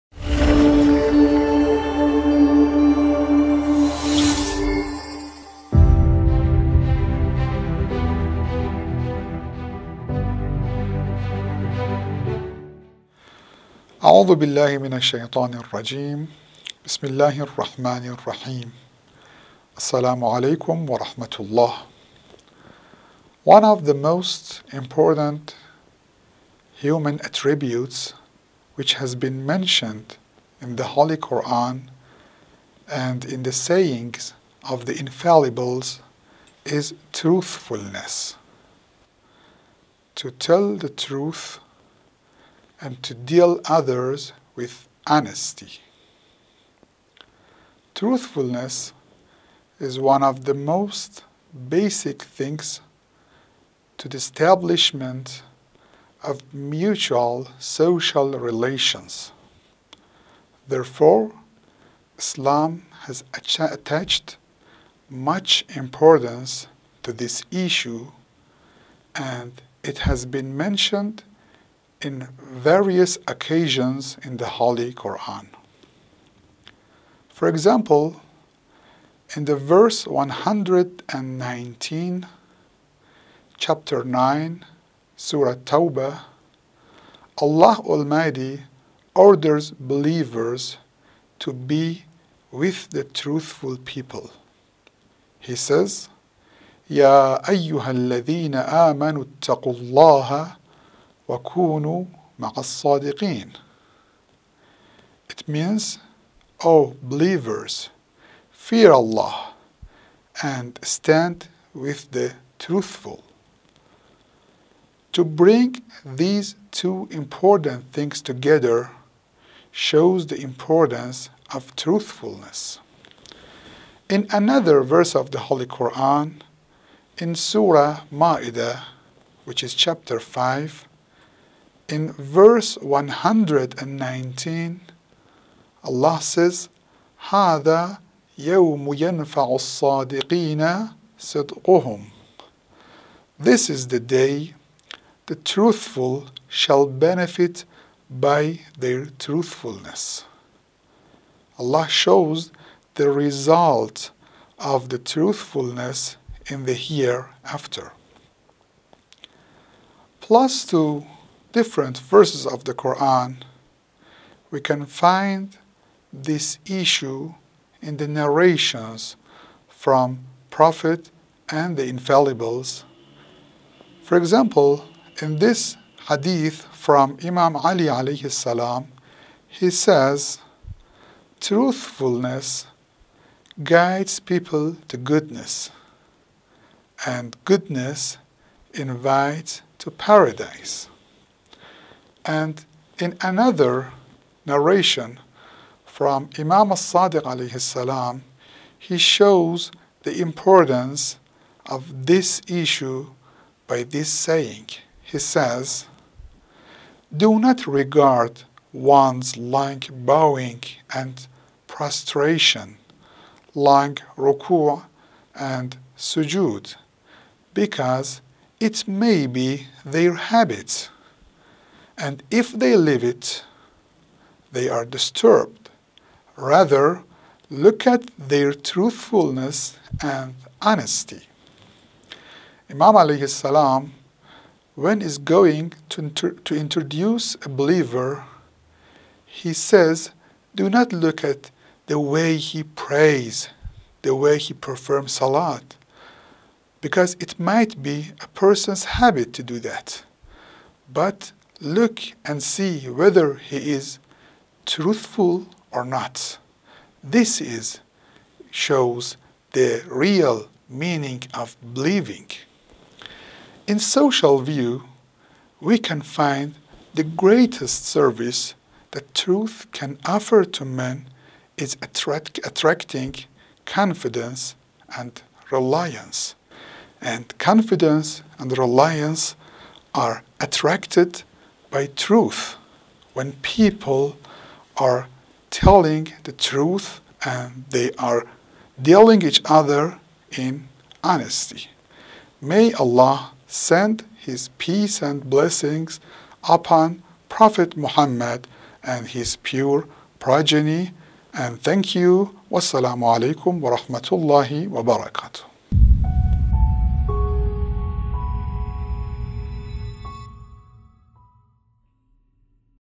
The Key to The Paradise
Truthfulness is one of the most basic and fundamental things to the establishment of mutual social relations, and its importance is to the extent that it has been mentioned as a criterion for recognizing someone as a true believer, therefore it has been mentioned in various verses and hadiths. In this lecture, you can learn about some of these verses and hadiths and their interpretation.